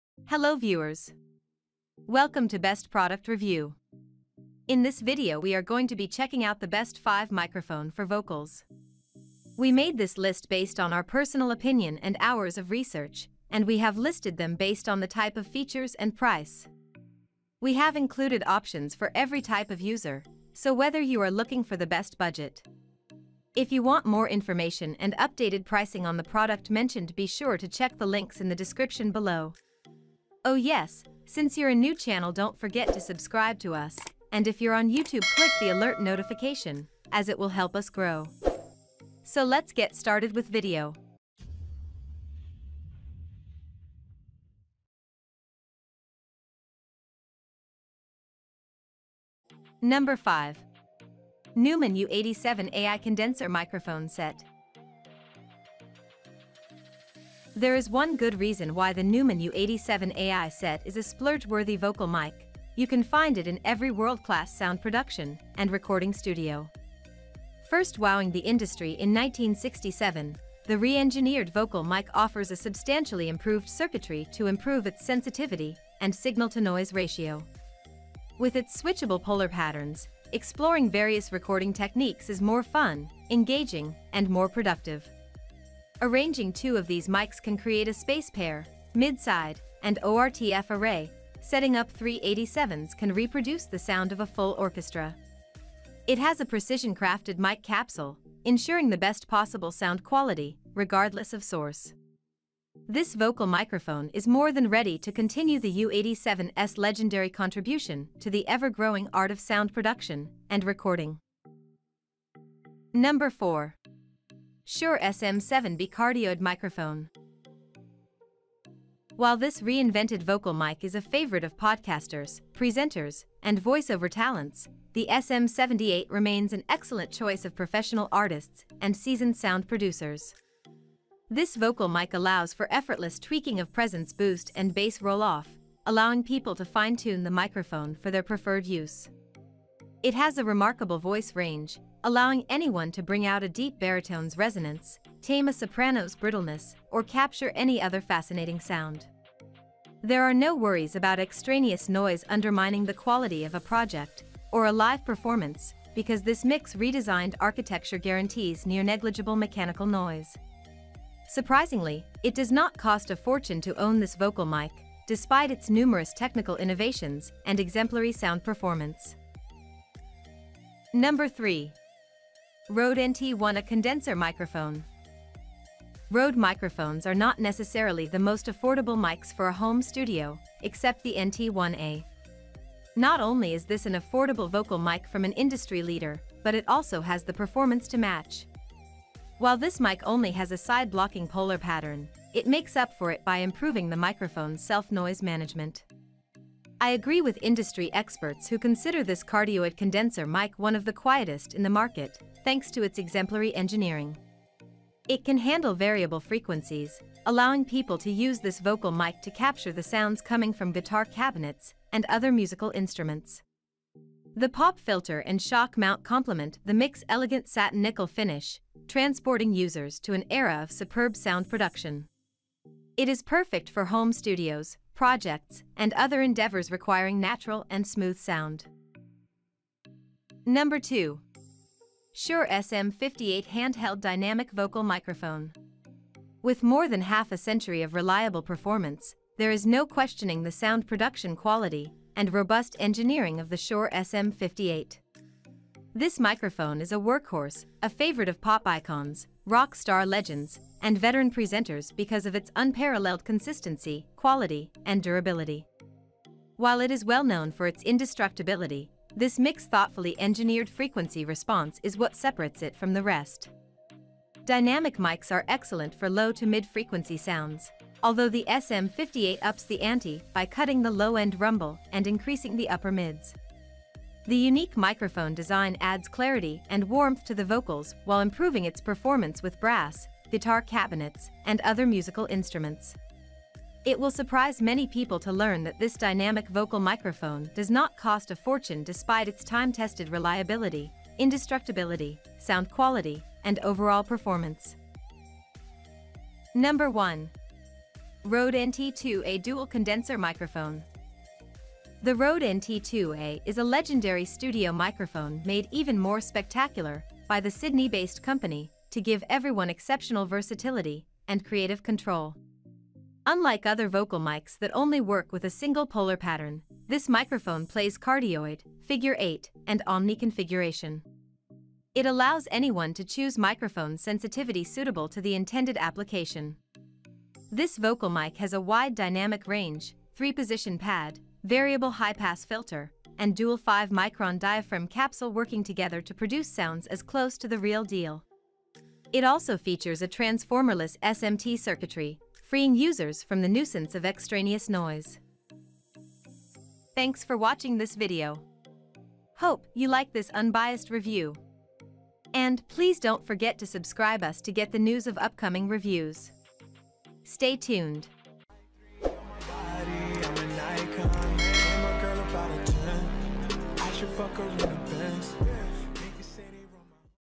✅ Top 5:🎤 BEST Microphone For Vocals In 2024 [ Best condenser mic for vocals ]
🎤In this video we are going to be checking out the best 5 Microphone For Vocals Links in below to pick the Best Microphone For Vocals.

1.Rode NT2A Dual Condenser Microphone ✅
✓ Amazon Price
2.Shure SM58 Handheld Dynamic Vocal Microphone ✅
✓ Amazon Prices
3.Rode NT1-A Condenser Microphone✅
✓ Amazon Prices
4.Shure SM7B Cardioid Microphone✅
✓ Amazon Prices
5.Neumann U 87 Ai Condenser Microphone Set✅
✓ Amazon Prices

🎤 All above  Best Microphone For Vocals in 2022 links are Applicable in USA, Canada, UK & Europe 🎤
➤ Don't forget to: